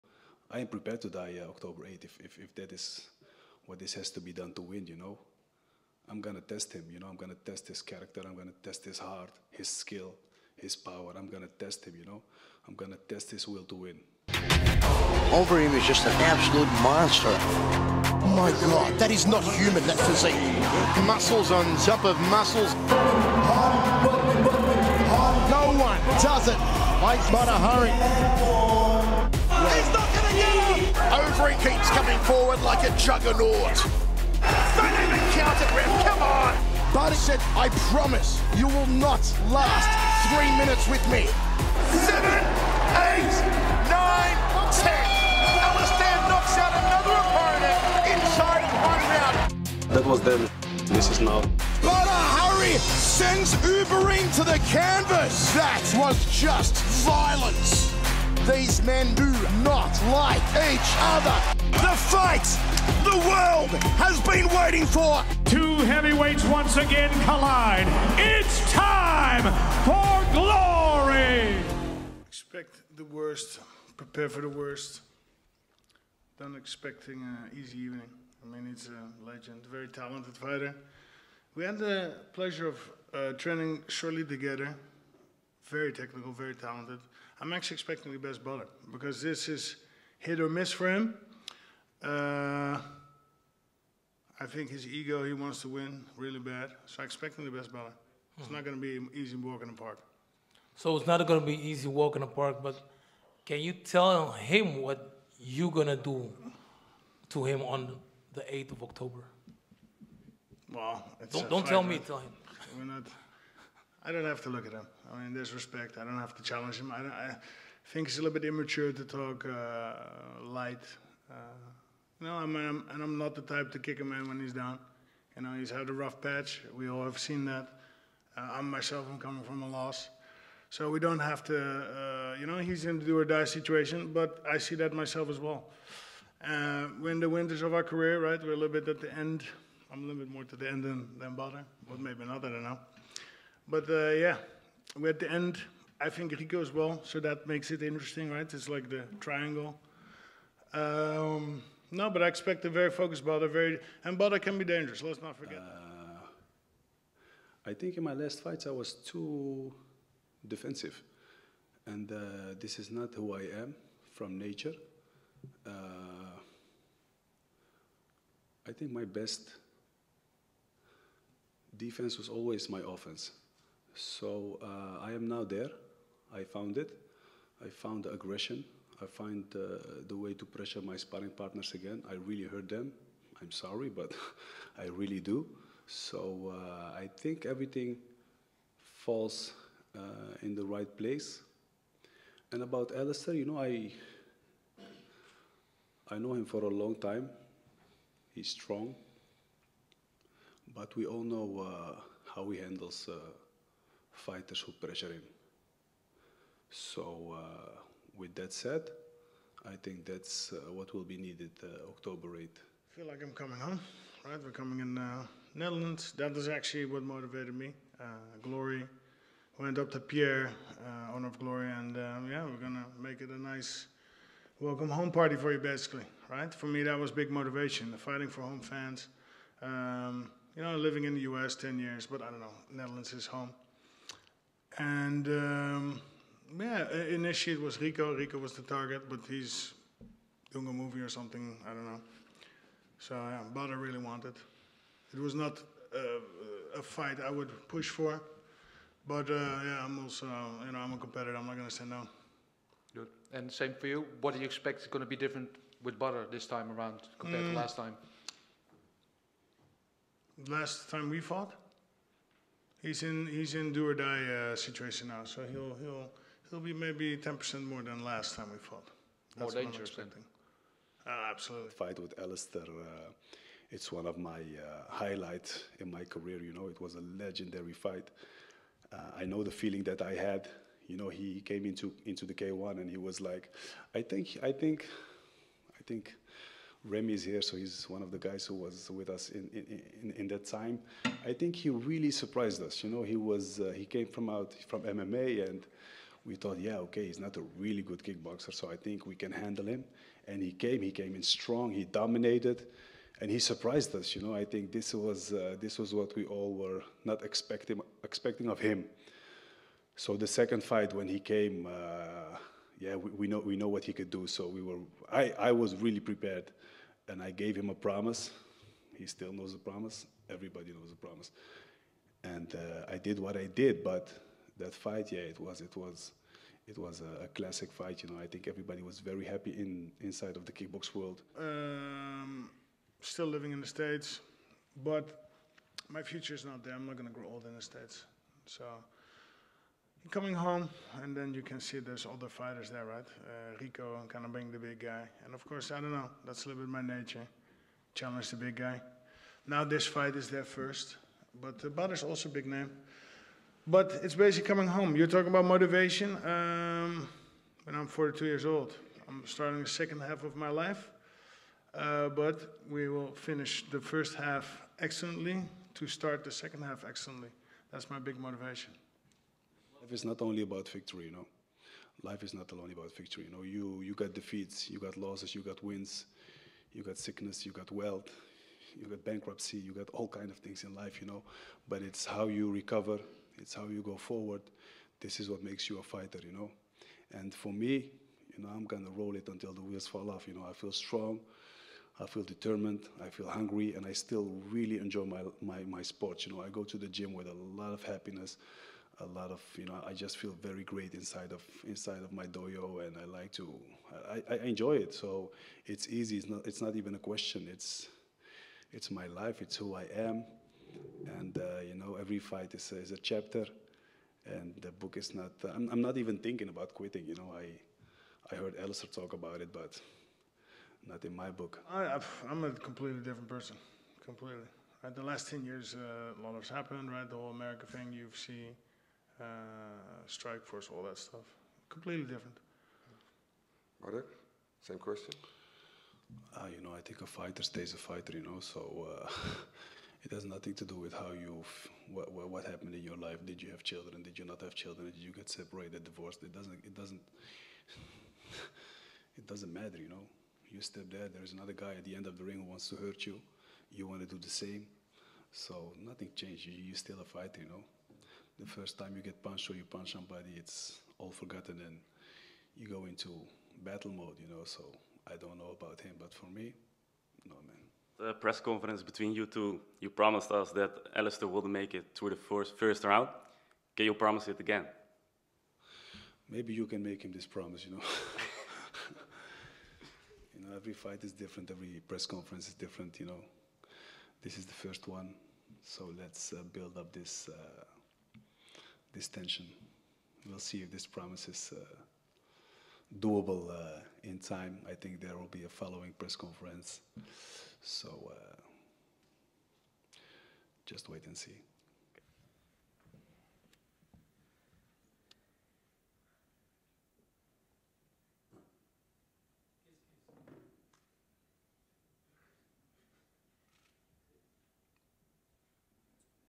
COLLISION 4 Press Conference Highlights
Watch the best moments from the COLLISION 4 Press Conference featuring Badr Hari and Alistair Overeem.

Tickets to COLLISION 4 are available at tickets.glorykickboxing.com